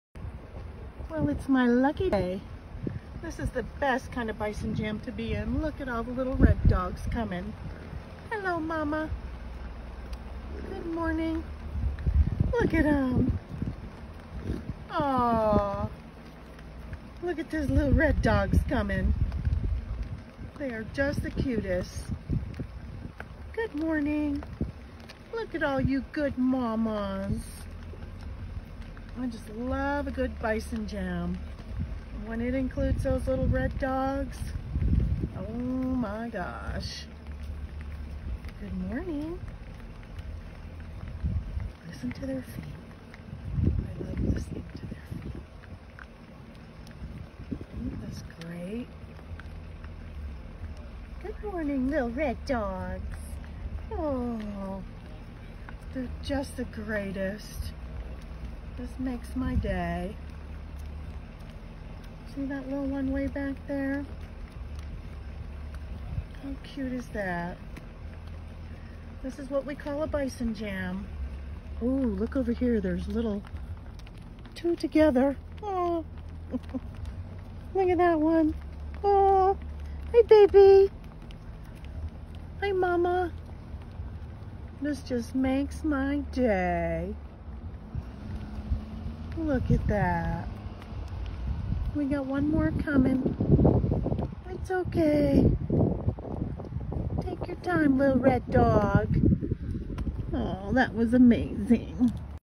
Roaming Bisons and Their Babies Halt Traffic in Yellowstone
Credit: Cindy Shaffer via Storyful